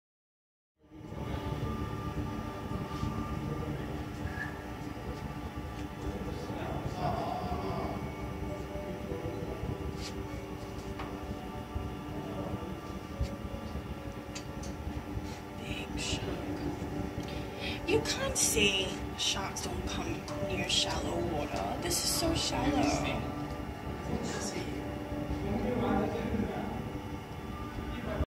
Sea World Giant turtle and sharks HD
Shot with Xperia Z2 at Seaworld Aus